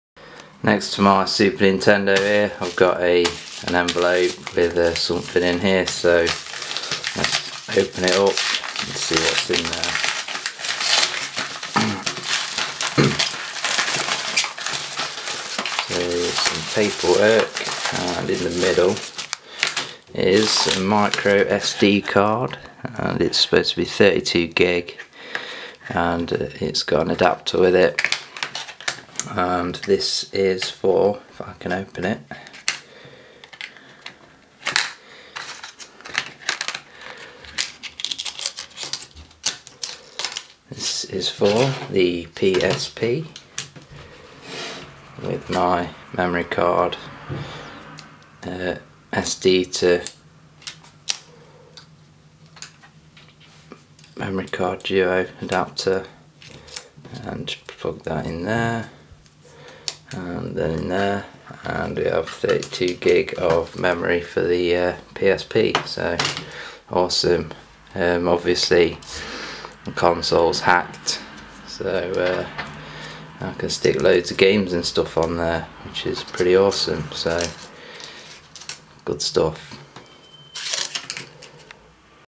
Read Description! 32gb Micro SD Card Unpacking For PSP - Classic Retro Game Room
DO NOT BUY A MEMORY CARD LIKE THIS FROM EBAY, THEY DO NOT WORK SELLER caa_tech_direct_ltd IS THE TRADER AND THESE CARDS ARE FAKE RUBBISH THAT DO NOT WORK!